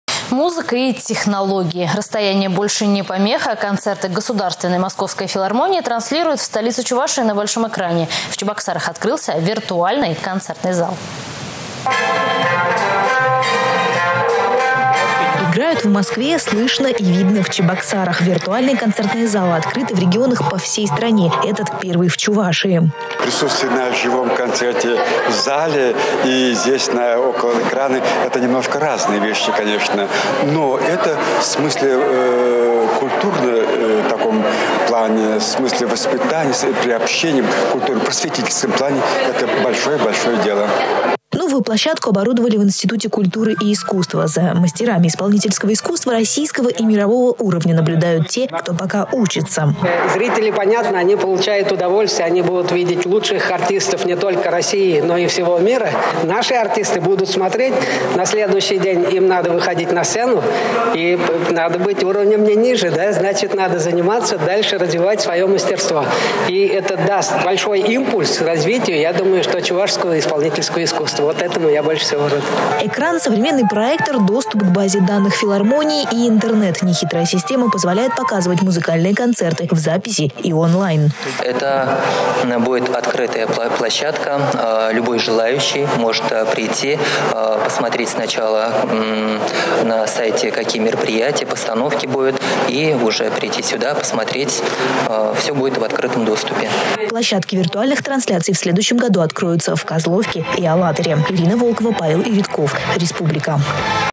В Чебоксарах открылся виртуальный концертный зал.
Музыка и технологии. Расстояние больше не помеха: концерты государственной московской филармонии транслируют в столице Чувашии на большом экране. В Чебоксарах открылся виртуальный концертный зал.